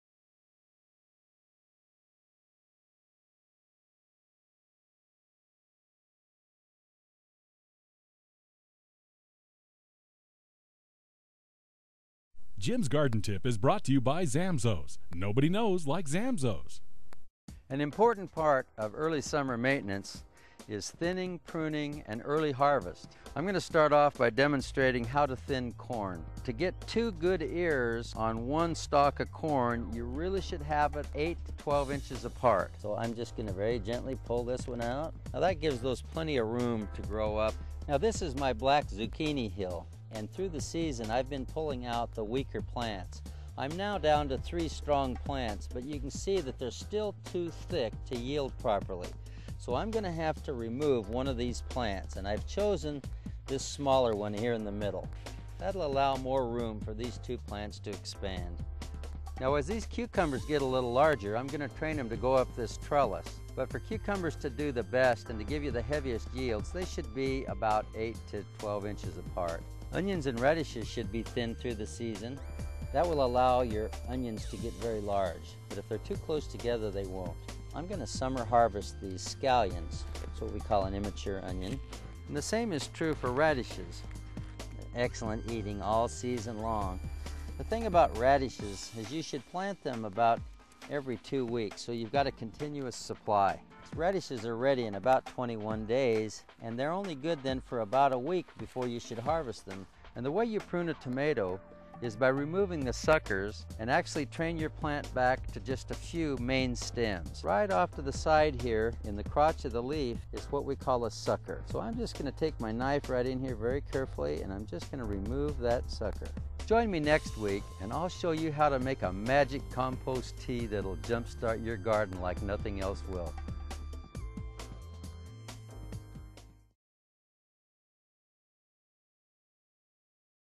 Jim Zamzows Gardening Tip # 10
Jim Zamzow shows you how to thin corn for maximum harvest, thinning Zuchini pants for maximum harvest, and the largest yeilding Cucumbers, along with how to prune a tomato plants, harvest radishes and how to get two good ears on one stock of corn. You can find many of these products